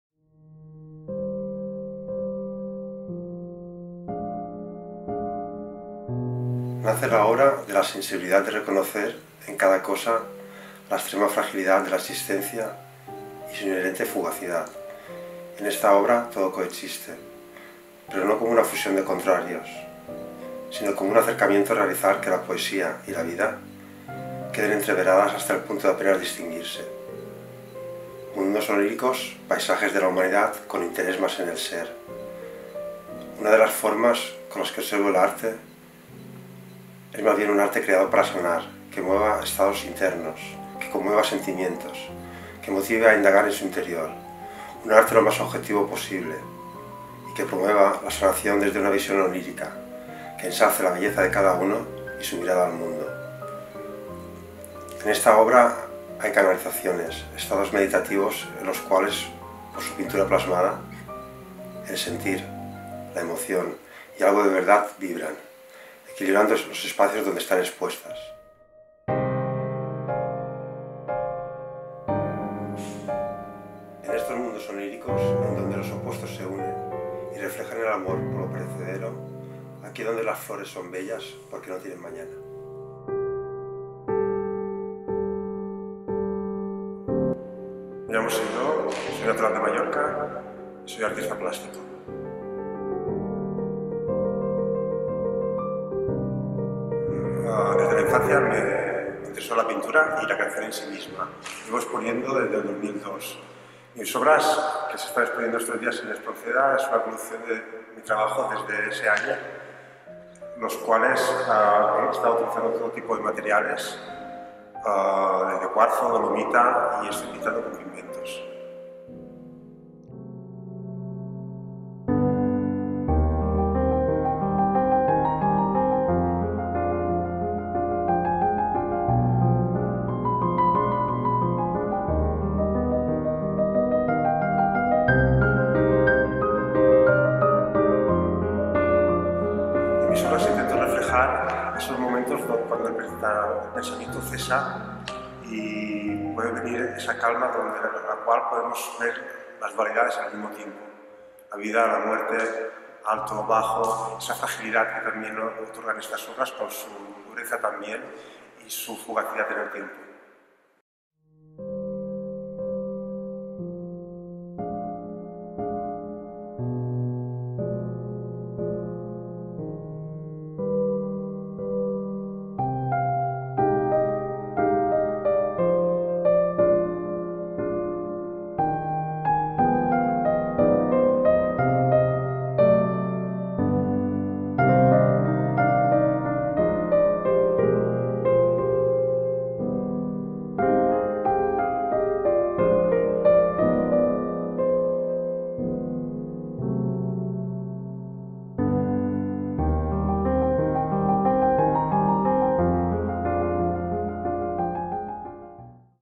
Exégesis Poética, by Siuro at Espronceda
This video is about Siuro interview during his artist residency at Espronceda - Institute of Art and Culture